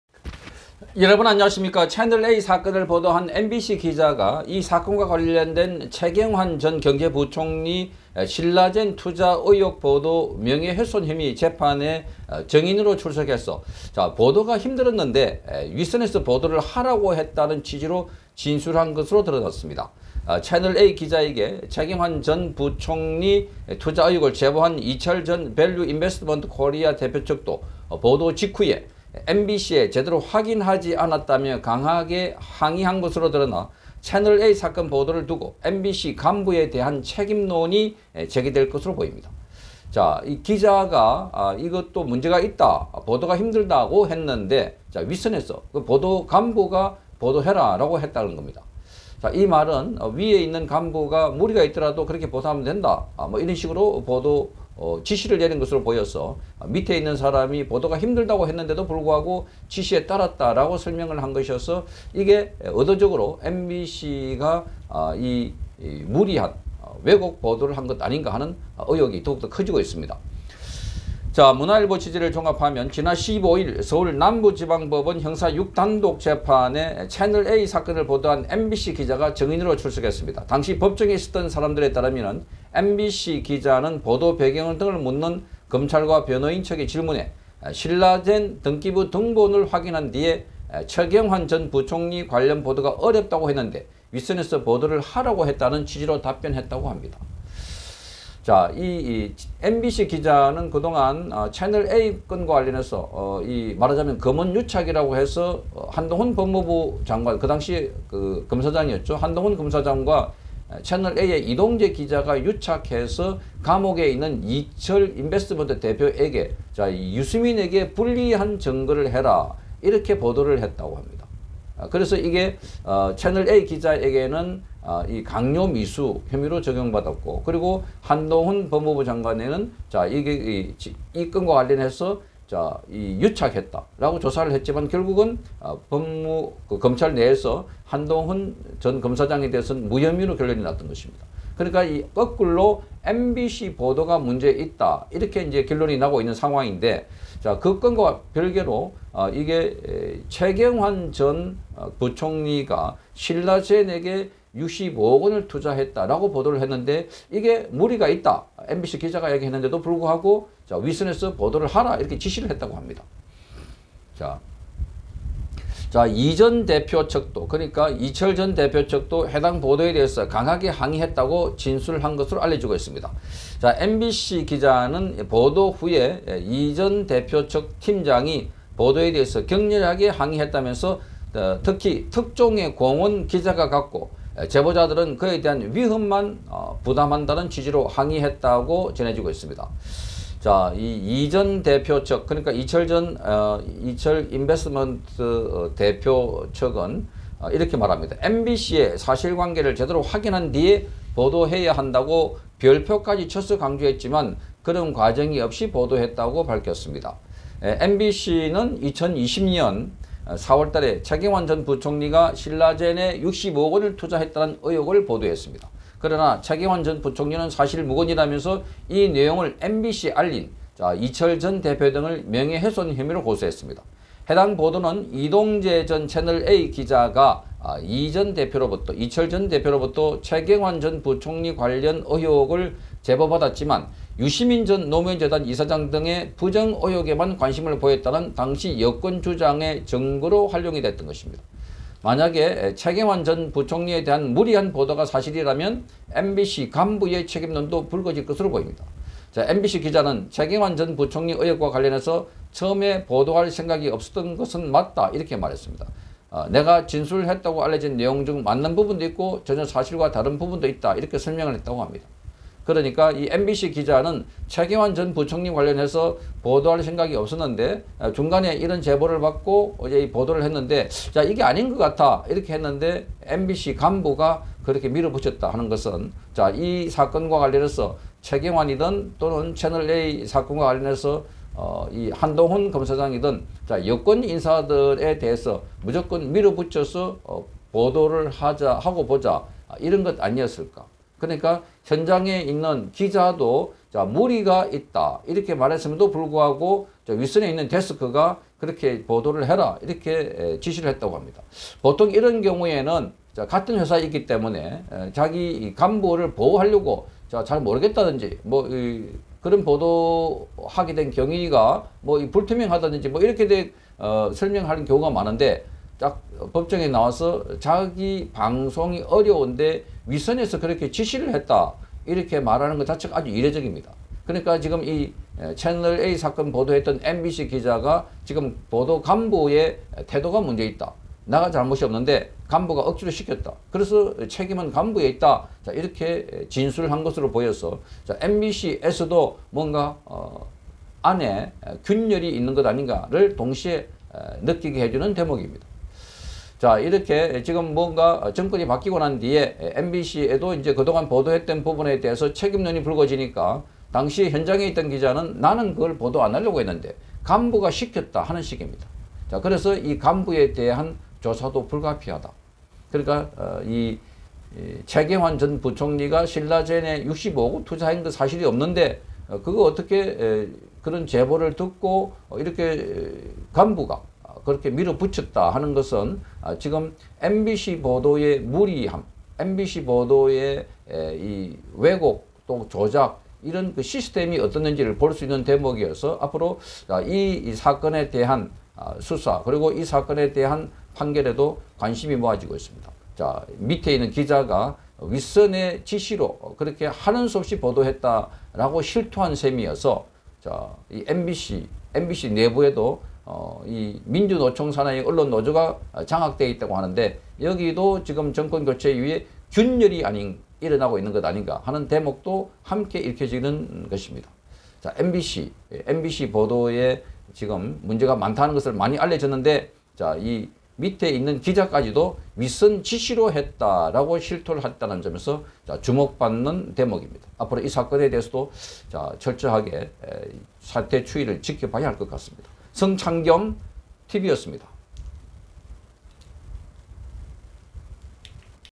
‘채널A 사건 보도’ MBC기자 법정출석 “방송 어렵다고 했는데 윗선에서 지시”
‘최경환 명예훼손’재판서 증언. 이철측 “사실확인 필요 말했다”
MBC 간부진 책임론 부상. MBC안에서도 간부와 일선 기자의 균열?